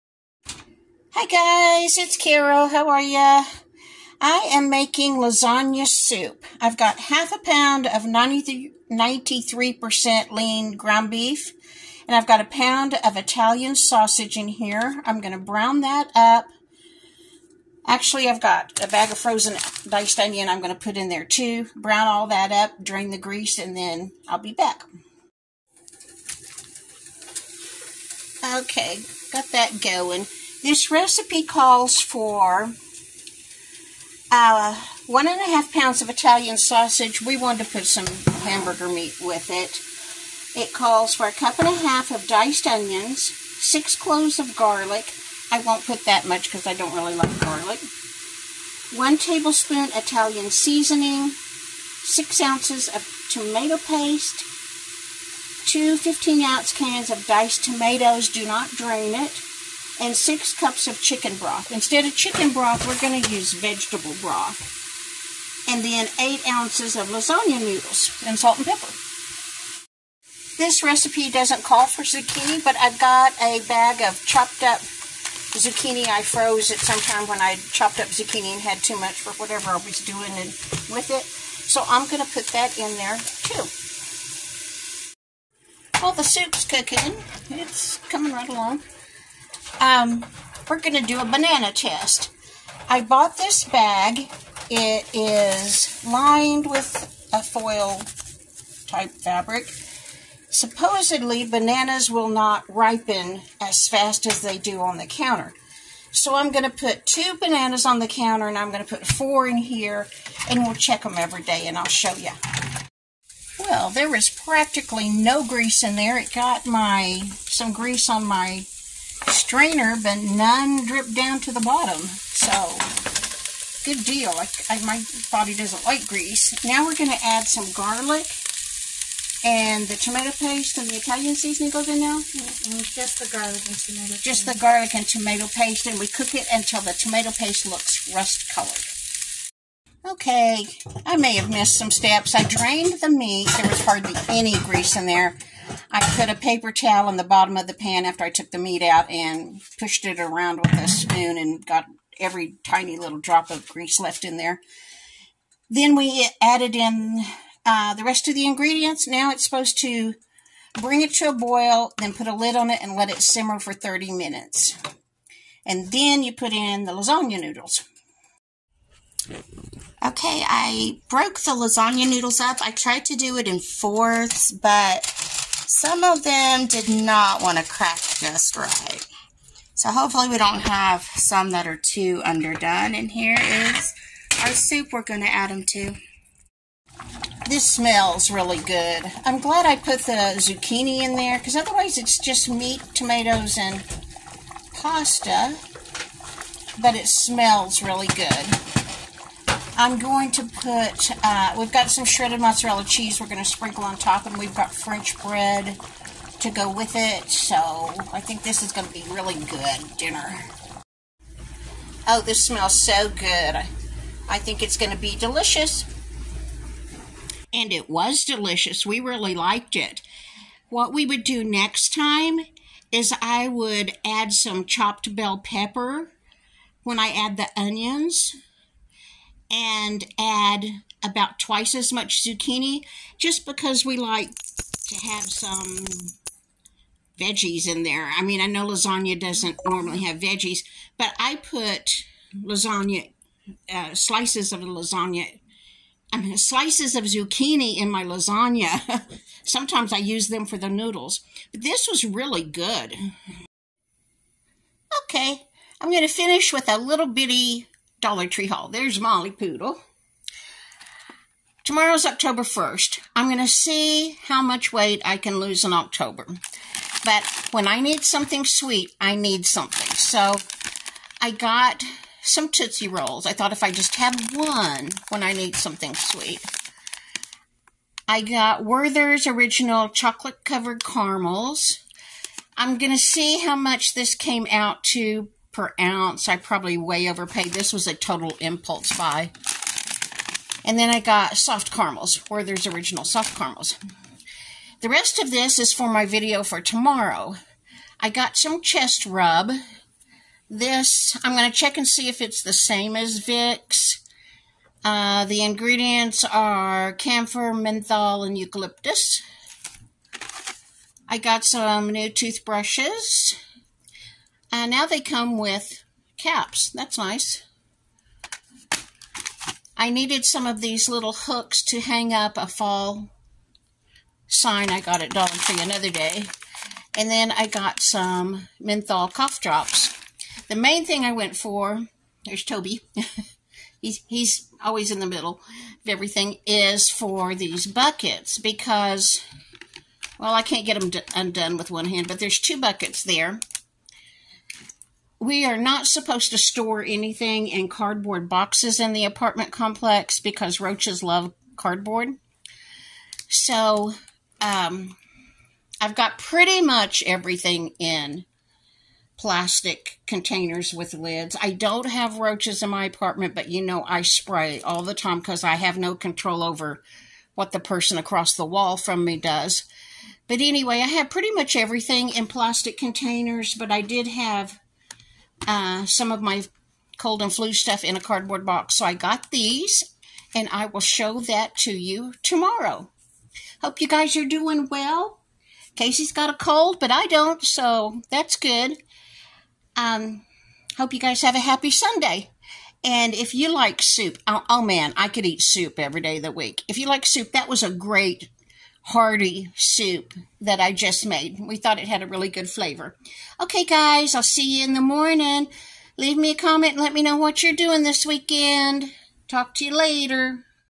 Lasagna Soup, A Banana Bag, And A Few Things From Dollar Tree
Lasagna Soup

1& 1/2 pounds Italian sausage
1 & 1/2 cups diced onion
6 cloves garlic, minced
1 Tablespoon dried Italian seasoning
6 oz can tomato paste
2 - 14.5 oz diced tomatoes, do not drain
6 cups chicken stock (I prefer vegetable broth)
8 oz lasagna noodles, broken

1.  Brown the sausage and onions, drain grease.
2. Stir in garlic, salt, pepper and Italian seasoning. Cook for 1 minute.
3. Add tomato paste. Stir and cook until the tomato paste becomes a deep rust color, about 3 minutes.
4. Add diced tomatoes and broth. (This is where I added my diced zucchini)
5. Bring to a boil. Reduce heat, cover and simmer 30 minutes.
6.  Add raw lasagna noodles. Cook until al dente.
7. serve with shredded mozzarella cheese on top.

NEXT TIME I WOULD ADD DICED BELL PEPPER WHEN I ADD THE ONIONS AND I WOULD ADD MORE ZUCCHINI.